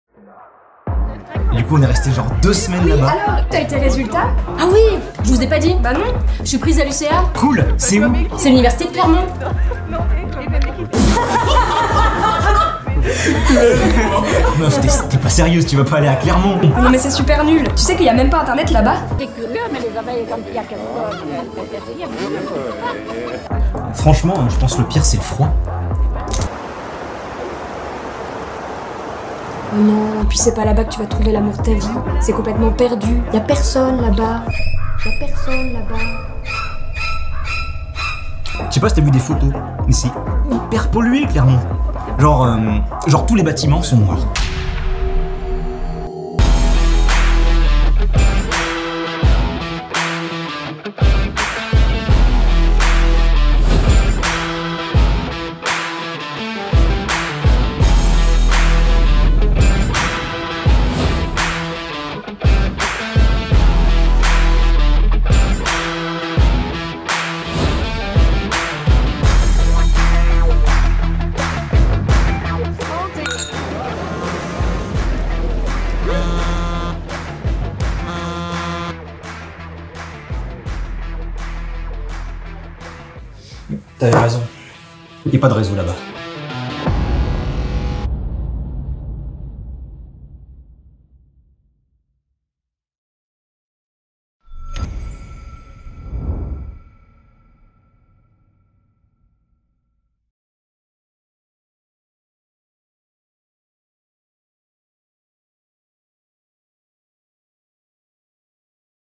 LA VÉRITÉ SUR CLERMONT !
On vous dit toute la vérité sur Clermont-Ferrand.

Les clichés ont la vie dure. Alors fais comme Iris, laisse tomber tes clichés et choisis une bonne université !

Réalisation : Service communication de l'Université Clermont Auvergne